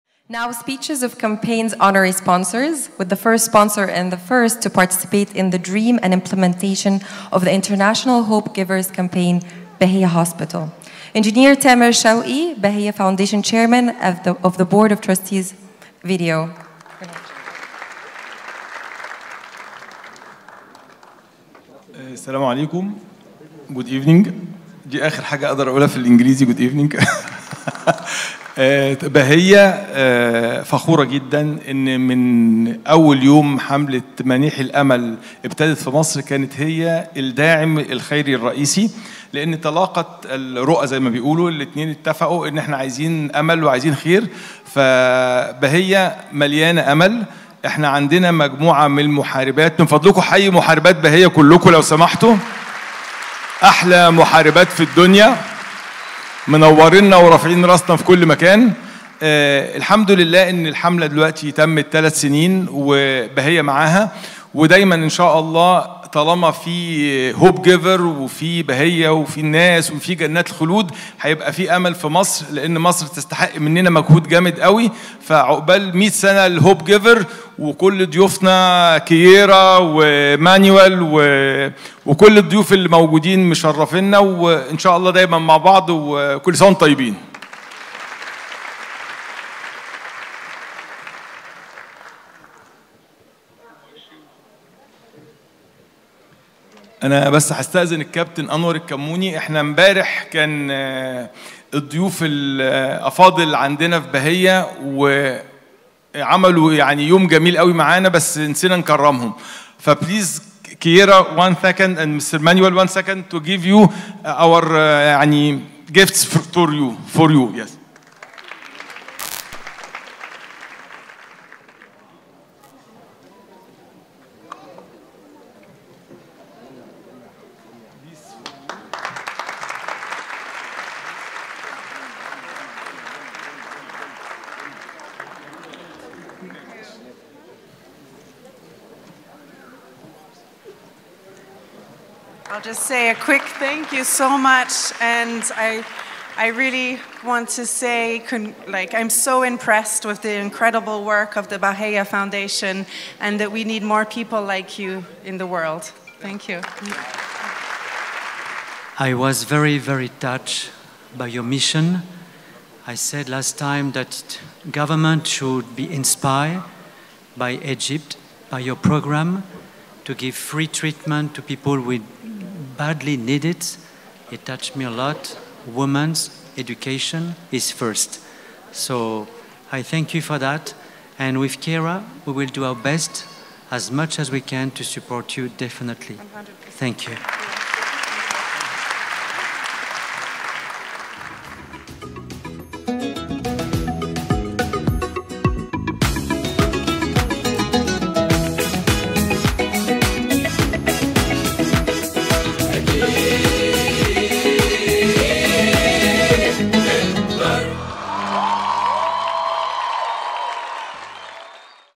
The Hope Giver Campaign | Speech of the Honorary Sponsor - Baheya Foundation (Closing 2023)
We would like to cordially thank our Honorary Sponsor, the biggest partner and supporter Baheya Foundation for presenting special awards to our esteemed guests - Manuel Collasdelaroche and Kiera Chaplin at the Closing Ceremony for the 3rd edition of the global Hope Giver campaign at MUST Opera House.✨🏆

The awards were presented by the board member of the Baheya Foundation - Eng. Maged Hamdy and delivered an inspiring speech to the audience. As well, we would like to deeply thank Eng. Tamer Shawky Baheya Chairman of the Board of Trustees for his presence and support. 🎗🙏